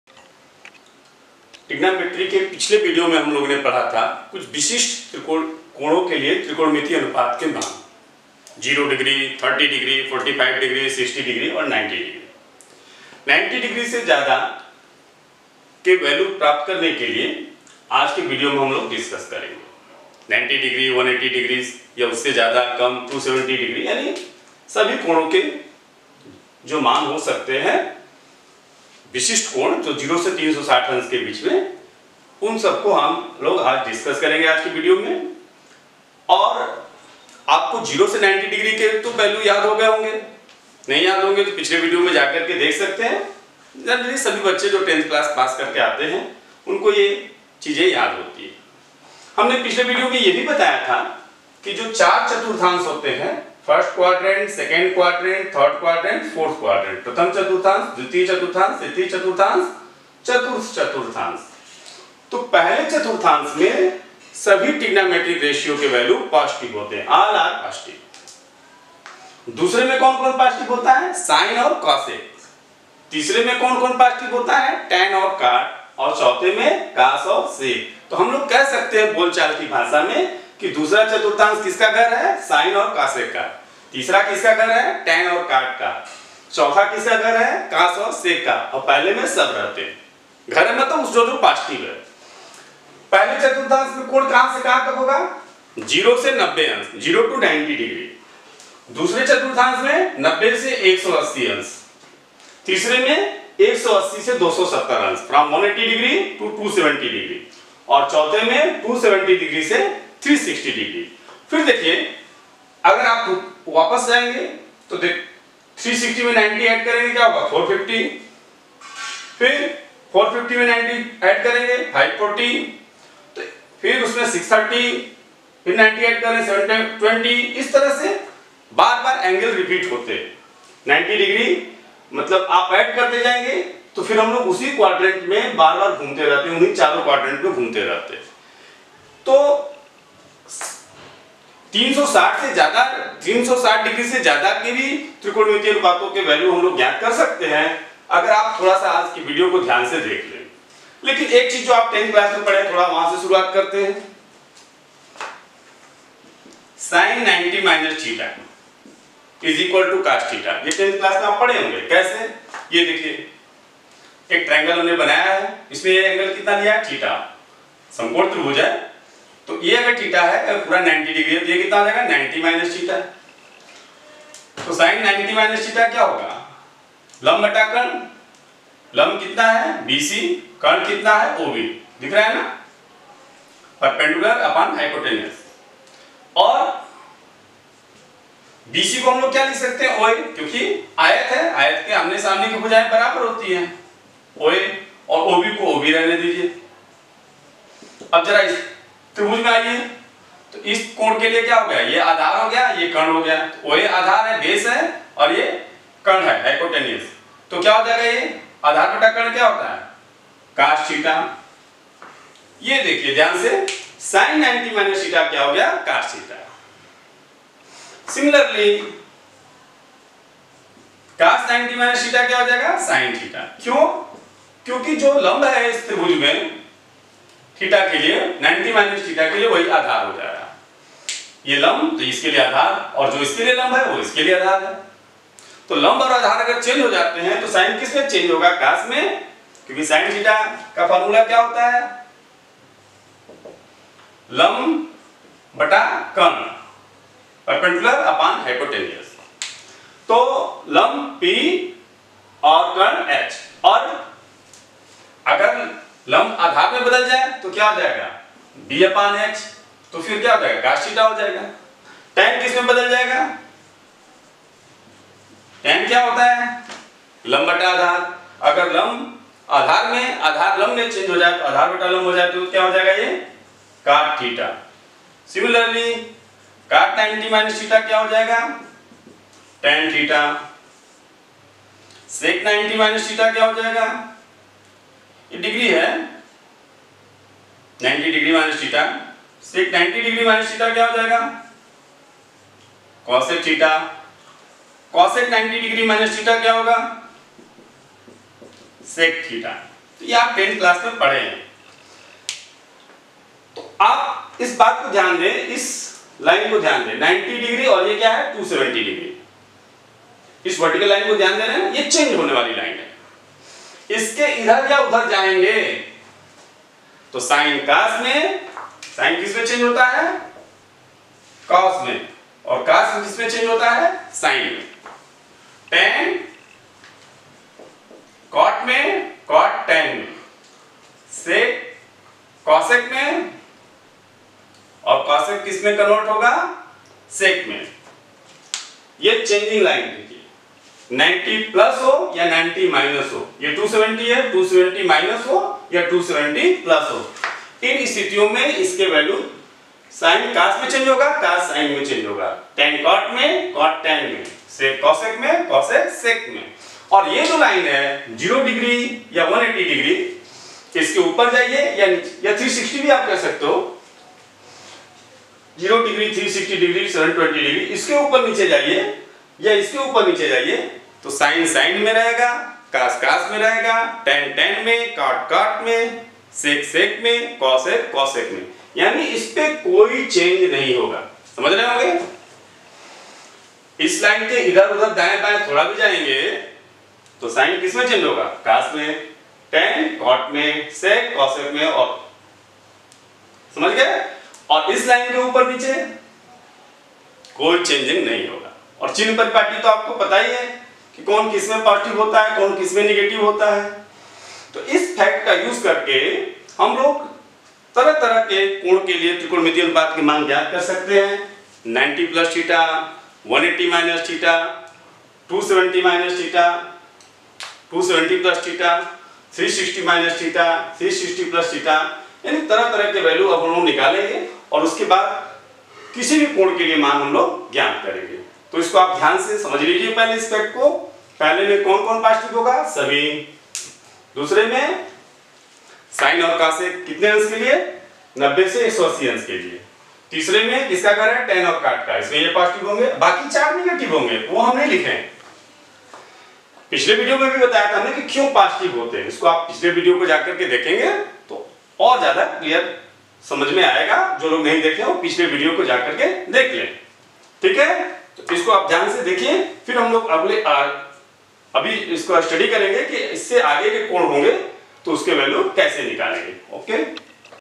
Basic Trigonometry (Lecture - 3 , Part 1) for class 11
You can easily find the value of trigonometric ratios for some specific angles greater than 90' (ninety degree).